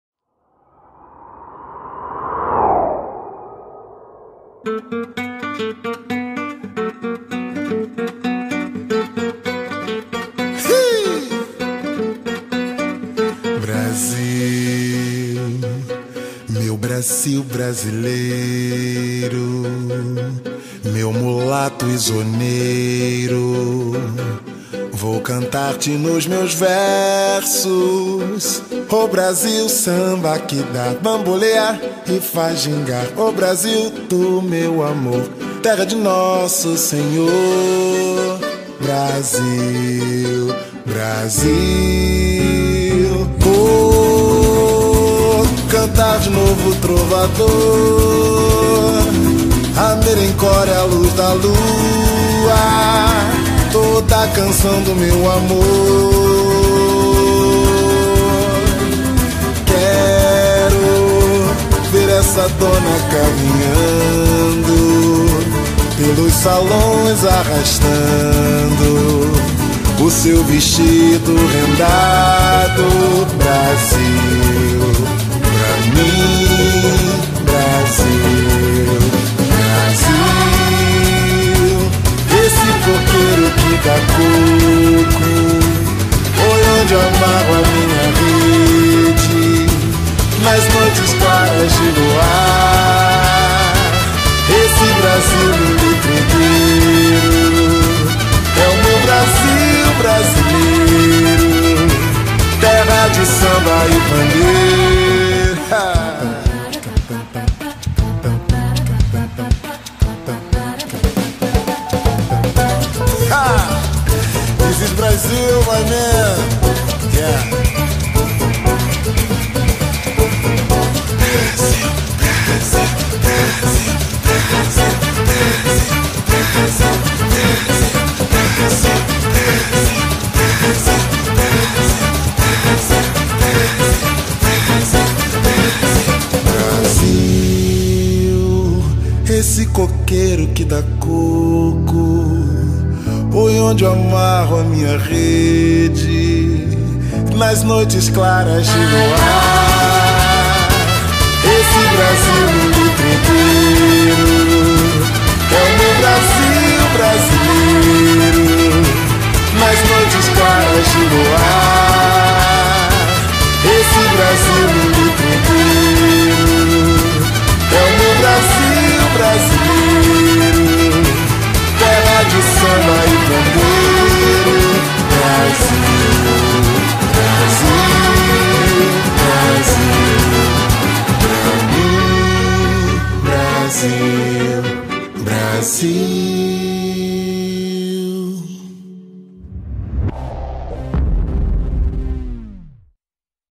RIO 2016 RIO DE JANEIRO, BRAZIL! COPACABANA & IPANEMA.
My visit to Rio De Janeiro, Brazil October 2015 in just over three minutes!